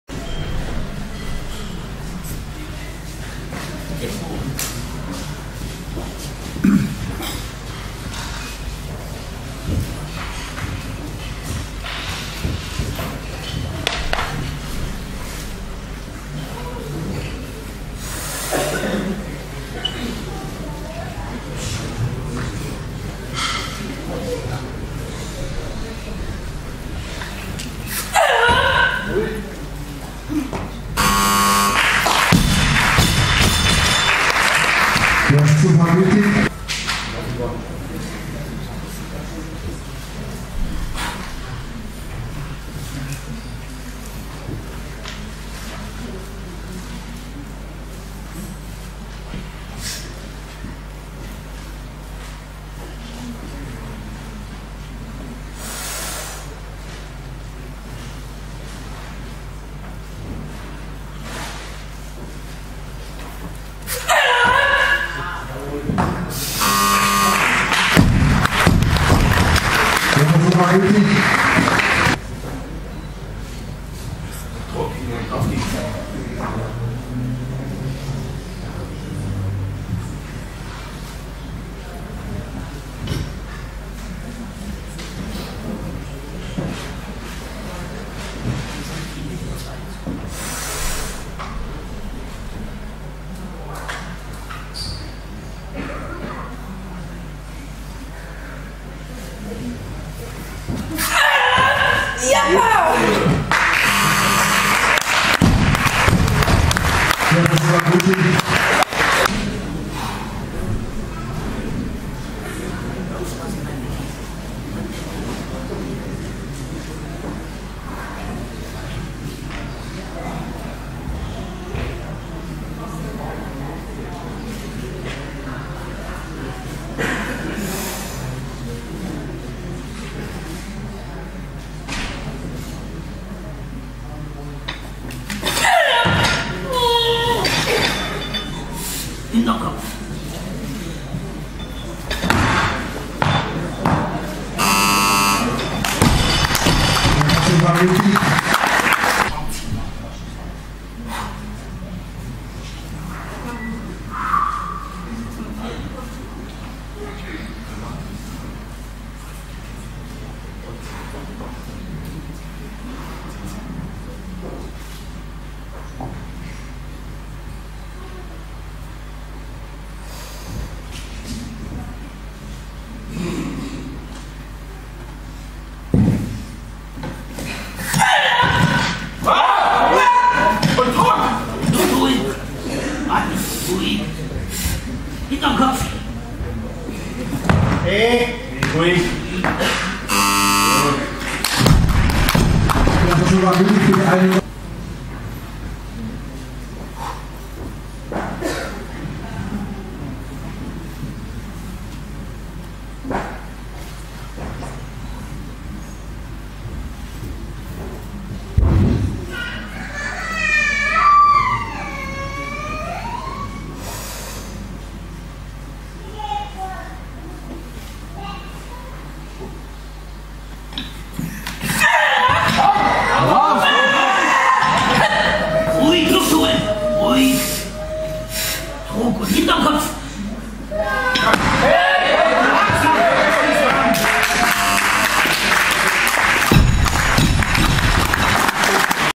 Yvonne Kranz - 2013-03-23 Herbert Ehrbar Turnier
Yvonne Kranz AC Suhl 1984 78,7kg: Reissen 95kg Stossen 124kg

Yvonne Kranz AC Suhl 1984 78,7kg: Snatch 95kg Clean and Jerk 124kg